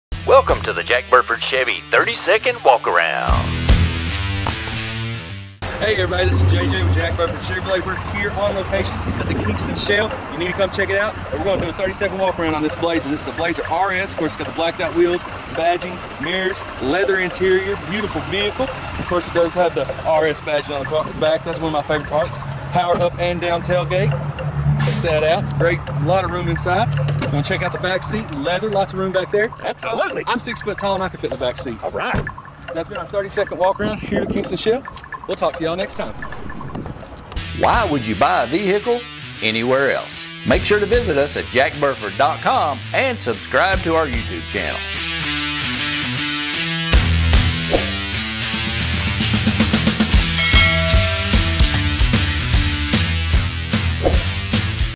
Blacked Out Chevy Blazer RS
Watch this 30 second walkaround on a blacked out Chevy Blazer - Click for more info about our Chevy Blazers folks - Message us to schedule your FREE TEST DRIVE!